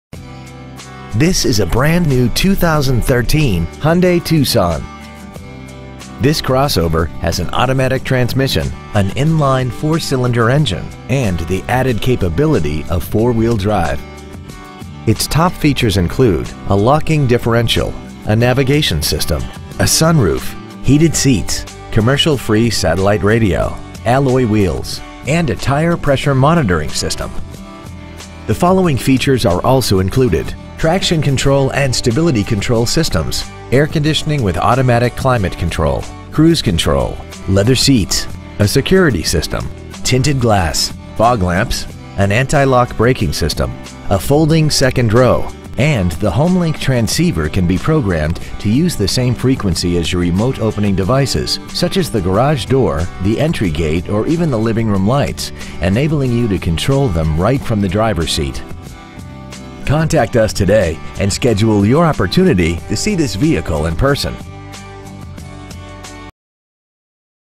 2013 Hyundai Tucson Kent WA
We are proud to present this 2013 Hyundai Tucson .

 Year : 2013
 Make : Hyundai
 Model : Tucson
 Engine : 2.4L I4 DOHC 16V
 Trans . : Automatic
 Exterior : Ash Black
 Miles : 25
 Interior : Black

 2925 Auburn Way N.

 2013 Hyundai Tucson Auburn WA